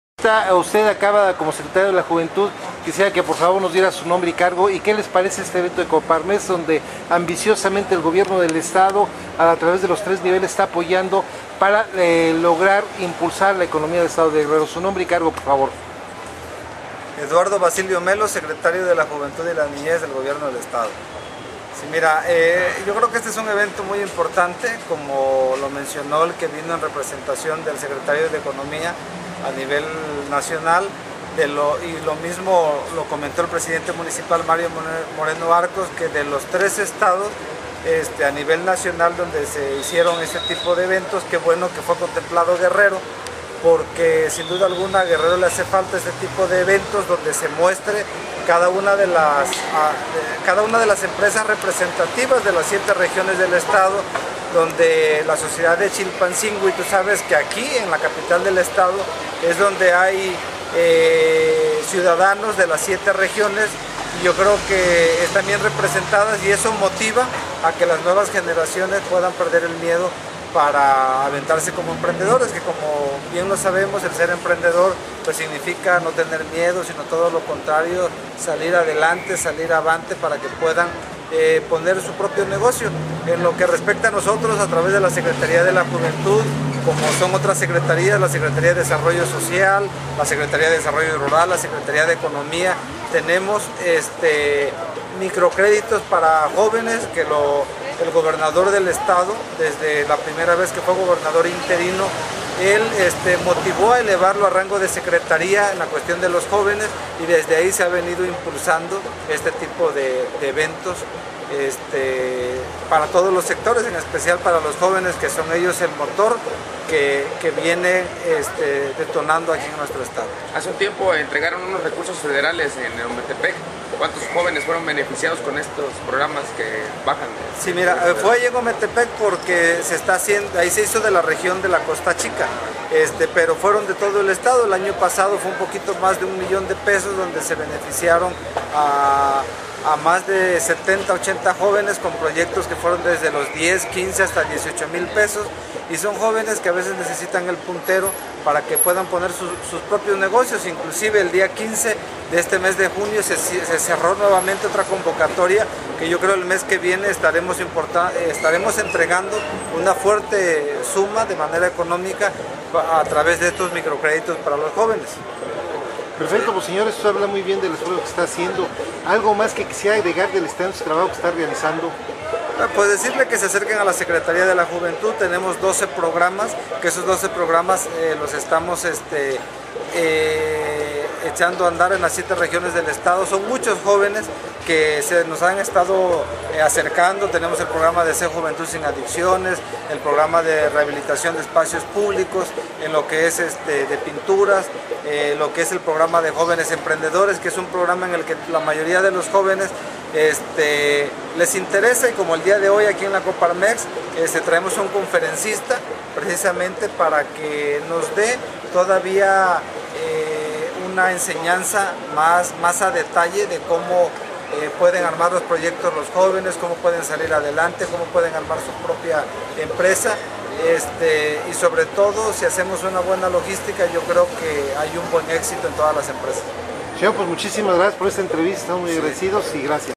Eduardo Bacilio Melo Secretario de la Juventud Platica de Feria de Coparmex en Guerrero
Eduardo Vacilio Melo Secretario de la Juventud Platica de Feria de Coparmex en Guerrero

transmisión continua las 24 Horas, los 365 días del año en forma permanente.

MOV02D